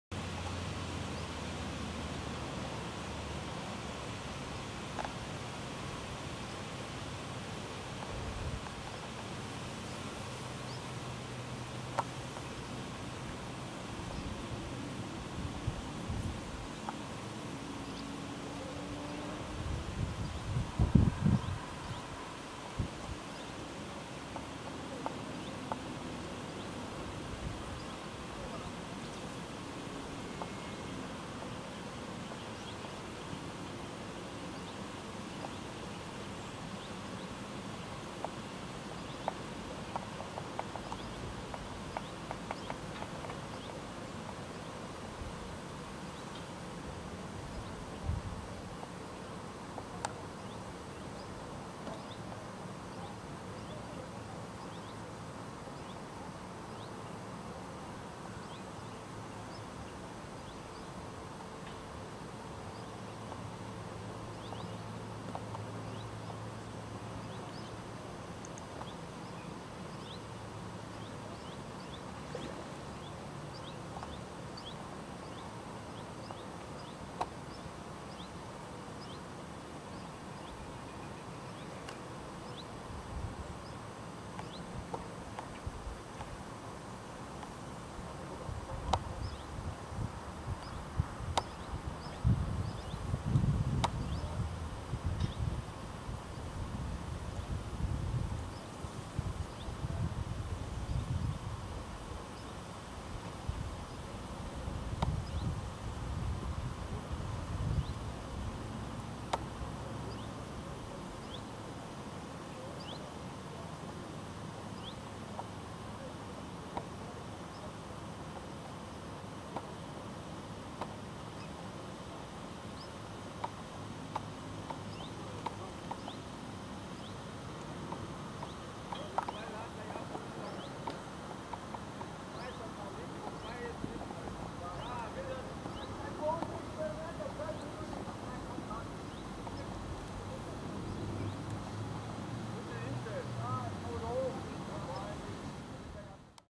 Kois im Schlossgraben - Nishikigoi in the castle moat (Royalty Free Stock Footage HD )
(Royalty Free Stock Footage HD )
Zwei Kois schwimmen entspannt im Schlossgraben ...
(Karpfen und Goldfische gibts auch dort)
Two Nishikigois in the castle mood .

Filmed with Camcorder Canon HV20